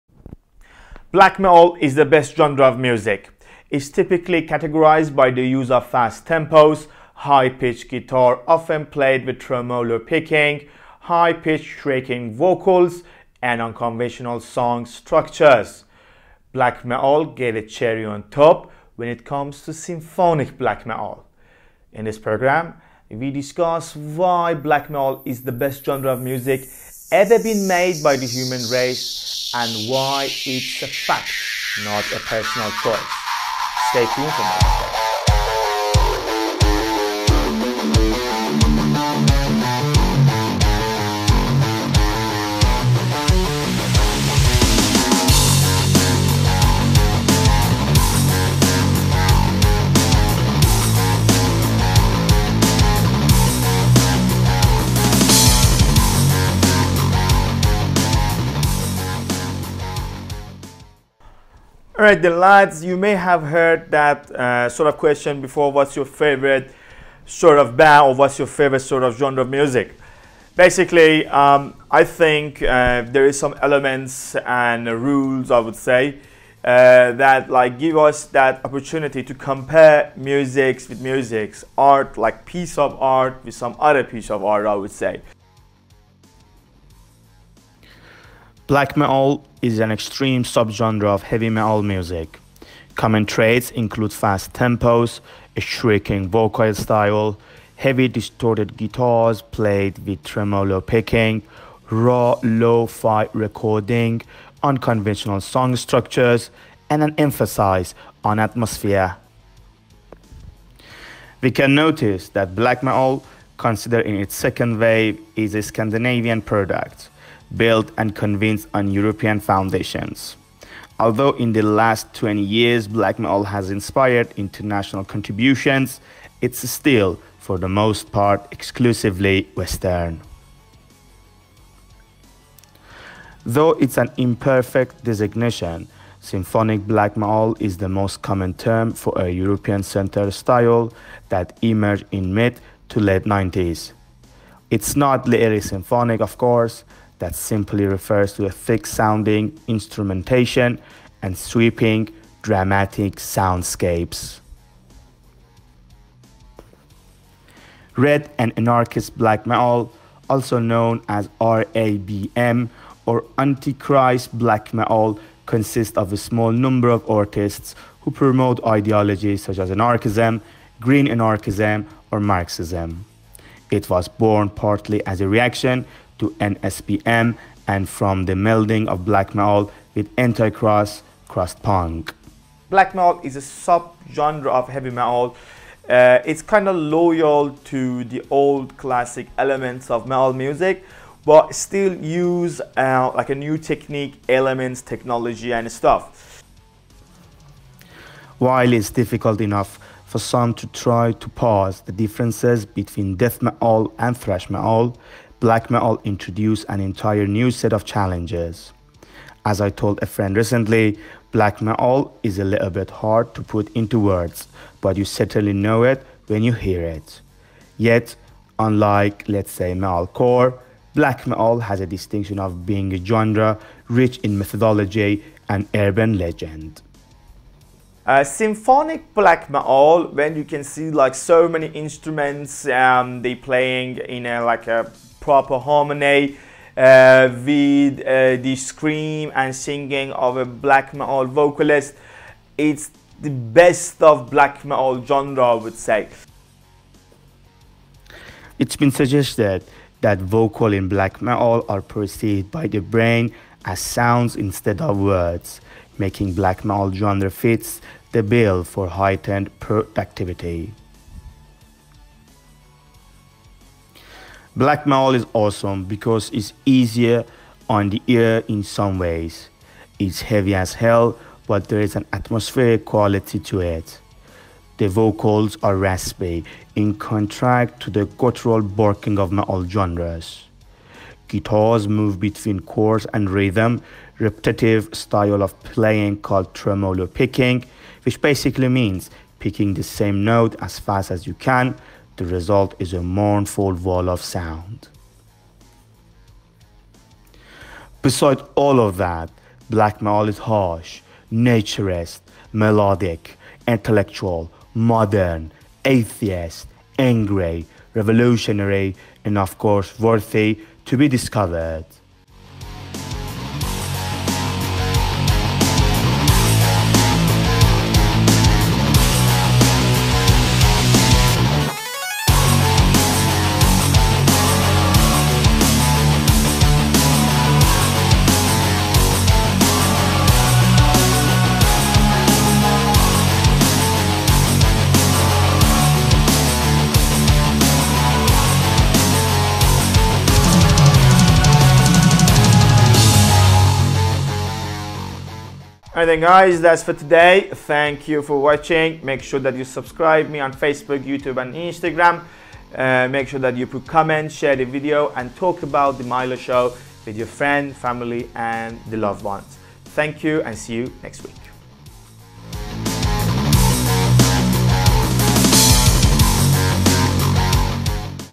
Black Metal is the Best Genre Ever Been Made-FACT
Milo Show:
It’s been suggested that the vocals in black metal are processed by the brain as sounds instead of words, making black metal genres fit the bill for heightening productivity.